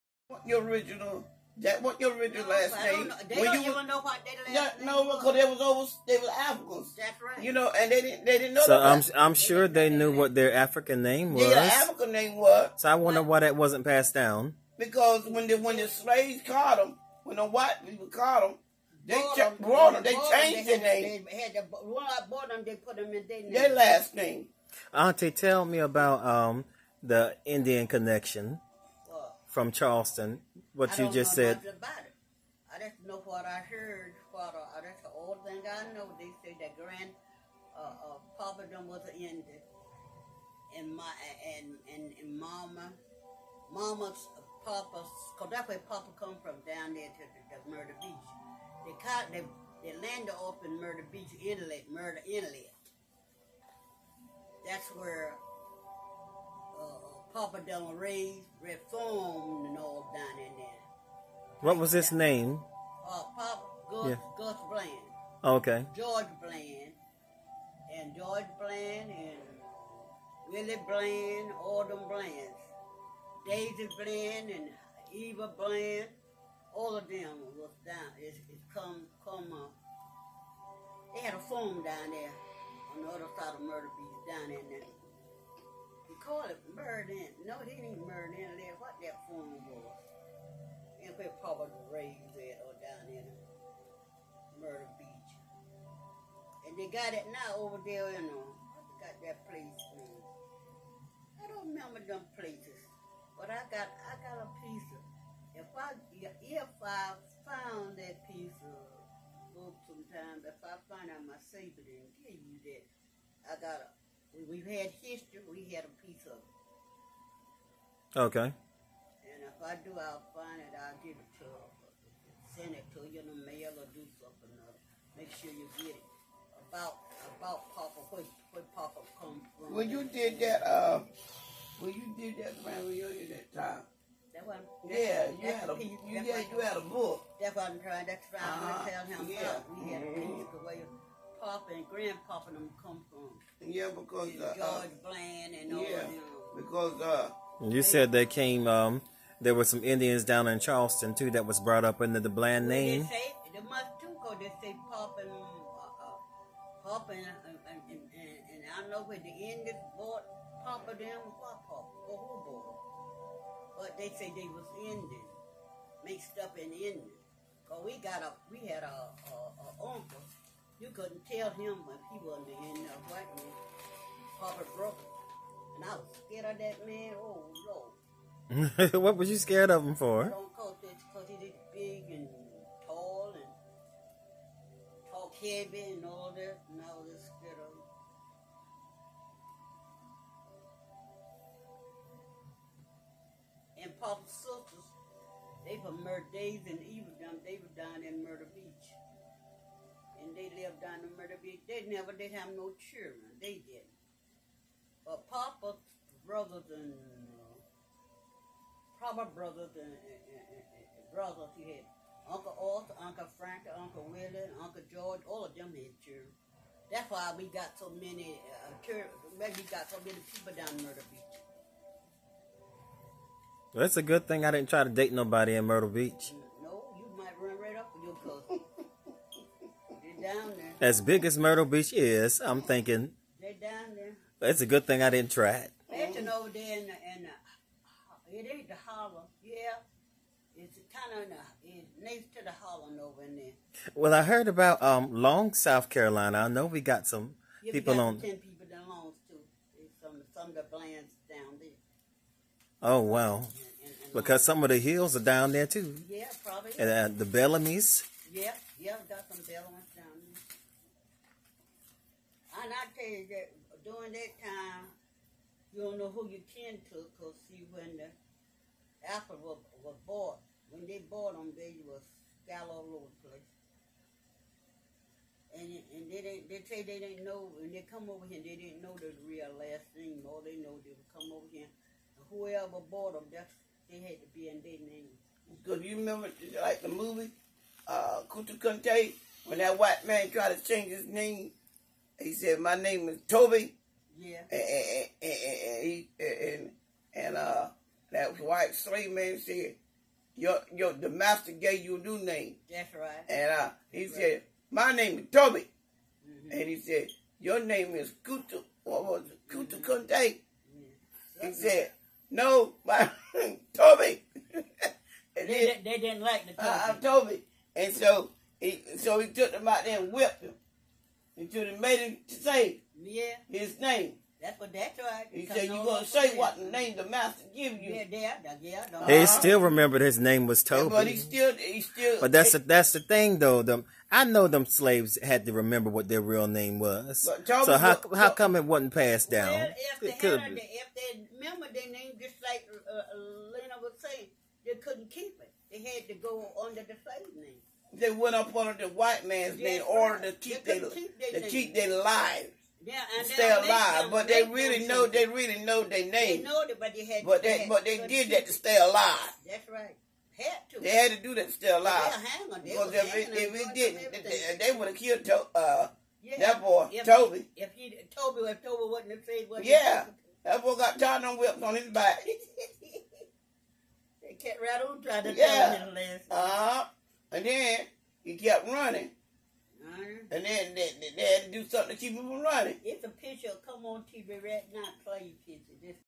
GULLAH GEECHEE/NATIVE AMERICAN CONNECTION
My family is mixed with West African and Indigenous American Indian Ancestry.
Aunt Florence Stowe and my mother(Sylena Hill) speak briefly about where the family migrated from.

Aunt Florence spoke of her Uncle Willie Bland(The Indian) and where the Bland name came from.

Bland last name was given to the slaves when they were purchased by the Bland family after  coming off the ship in Charleston, SC.

HT HILL MUSIC & EDUTAINMENT LLC